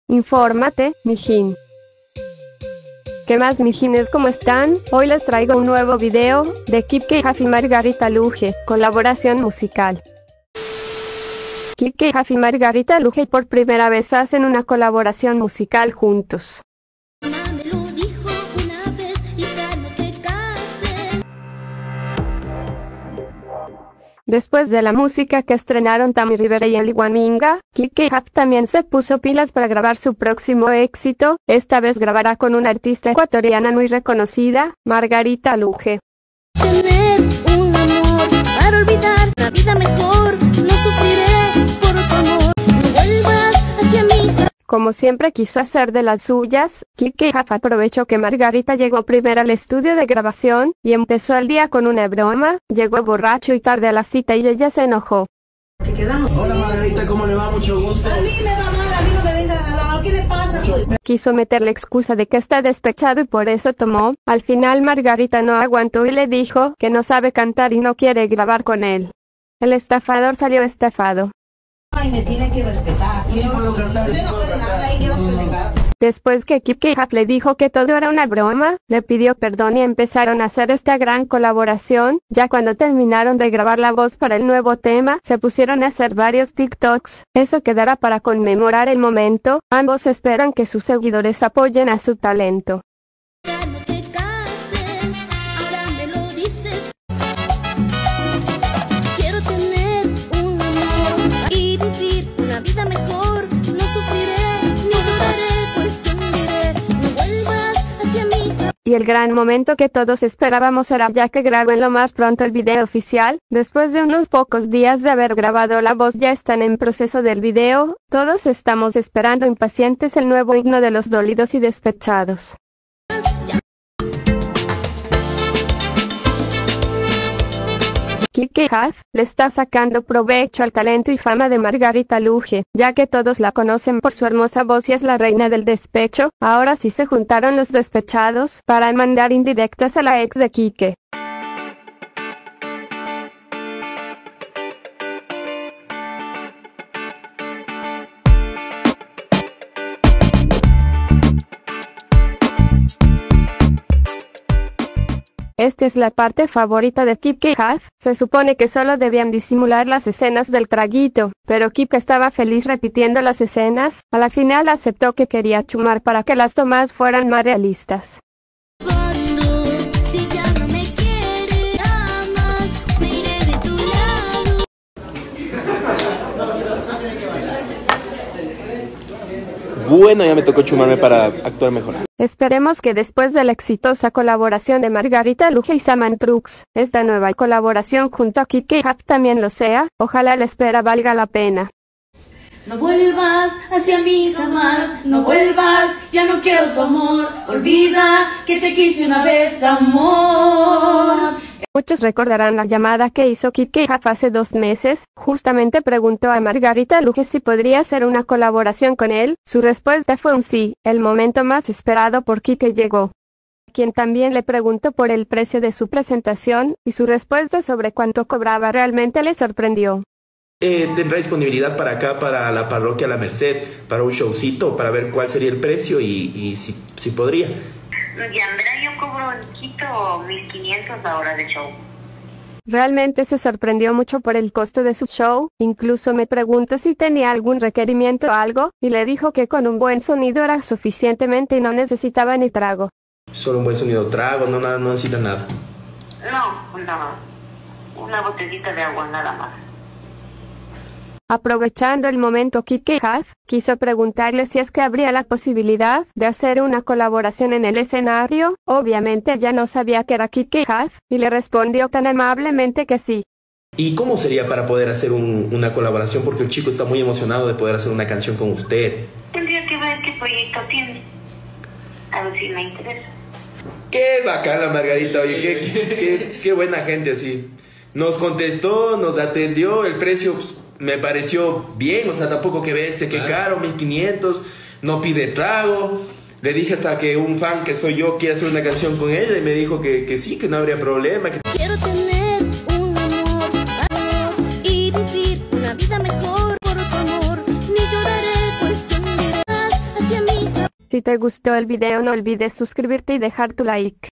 KIKE JAV Y MARGARITA LUGE COLABORACIÓN MUSICAL
INFÓRMATE MIJIN!!

En el siguiente video veremos a KIKE JAV, hace una colaboración con la artista Margarita Luge